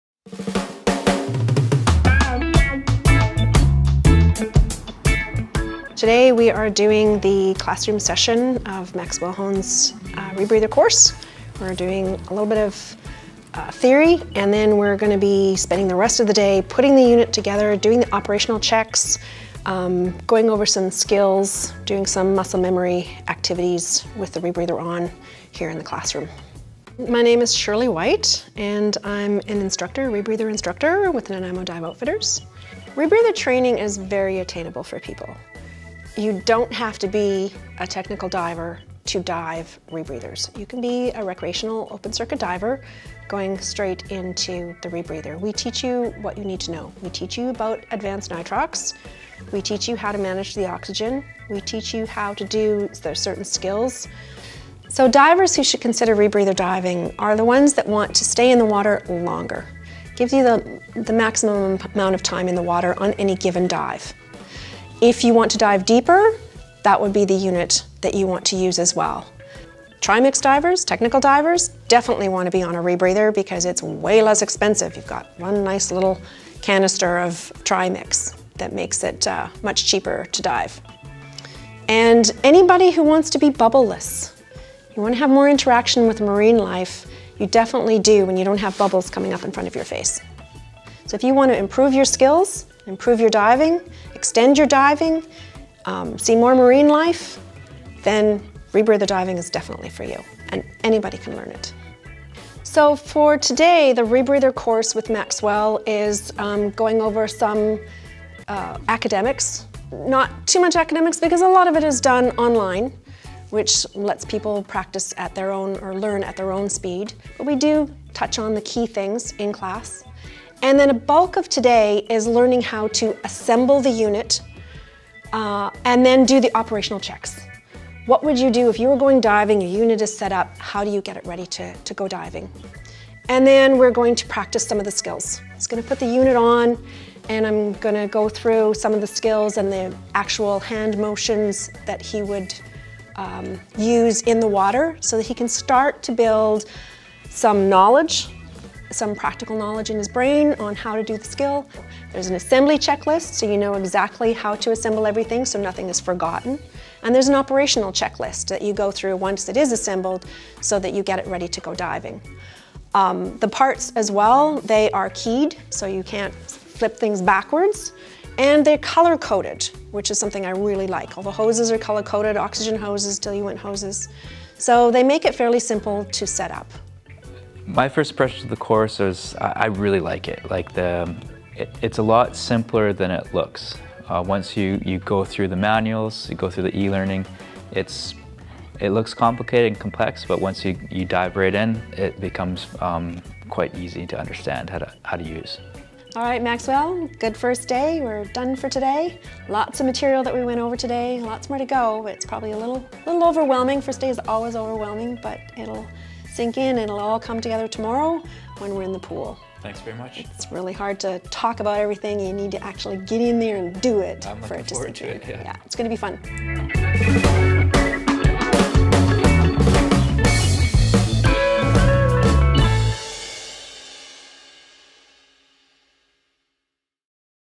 Inside Out Episode 2
In Episode 2, we familiarize ourselves with Maxwel’s new instructor and classroom as he attends his first day of training. He learns to set up his Prism 2, and reviews theory, operational checks and more.